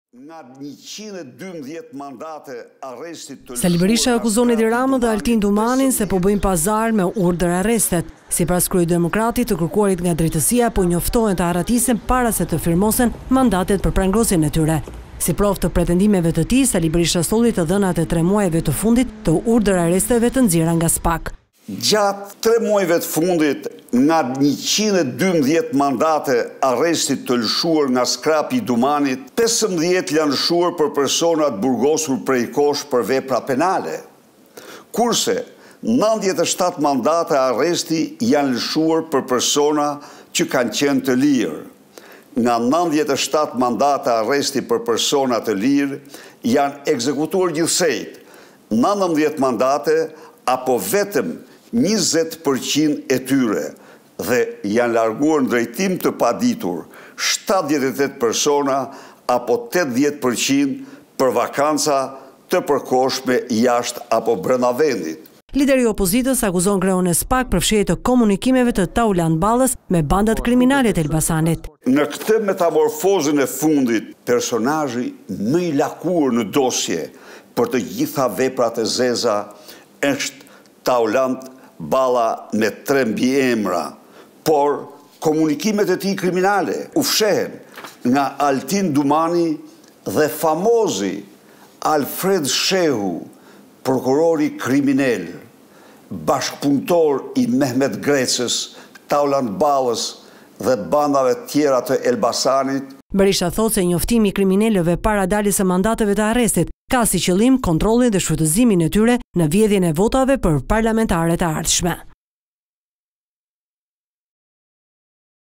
Berisha: 7 familje mafioze, të pathënat e dosjeve të SPAK, komunikimet në SKY - Vizion Plus
Ish-kryeministri Sali Berisha në një video-konferencë ka komentuar aksionet e fundit të SPAK, të cilat sipas tij kanë dështuar të gjitha.Në fjalën e tij Berisha theksoi se kriminelët dhe personat në kërkim, janë lajëmrauar të lëargohen nga vendi para se të dalë lista me urdhërarreste.Sipas tij nga 51 mandate të firmosura për metamorfozen 2 përveç 7 personave që kanë qenë në burg për vepra tjera penale kur janë lëshuar urdhër-arrestet, deri sot janë arrestuar 7 të tjerë kurse 36 u njoftuan të largohen në drejtim të paditur, ndër të cilët të gjithë bosët e 7 familjeve më të rrezikshme mafioze të Shqipërisë, Ballkanit dhe ndër më të rrezikshmet në Europë“Encrochat etj te bandave kriminale shqiptare, shërbimet partnere kane zbuluar qindra krime dhe vrasje trafiqe të bandave kriminale shqiptare të kryera në Shqipëri dhe jashtë vendit.  Lidhje të forta politike të të gjithë niveleve të qeverisjes përfshi këtu Edi Ramën. Me këto dokumente, regjistrime të transkriptuara të një rëndësie kapitale për luftën kundër krimit dhe korrupsionit, Spaku i Altin Dumanit po bën pazare të mëdha. Kështu nga 51 mandate të firmosura për metamorfozen 2 përveç 7 personave që kanë qenë në burg për vepra tjera penale kur janë lëshuar urdhër-arrestet, deri sot janë arrestuar 7 të tjerë kurse 36 u njoftuan të largohen në drejtim të paditur, ndër të cilët të gjithë bosët e 7 familjeve më të rrezikshme mafioze të Shqipërisë, Ballkanit dhe ndër më të rrezikshmet në Europë të gjitha të lidhura me Edi Ramën”, tha Berisha.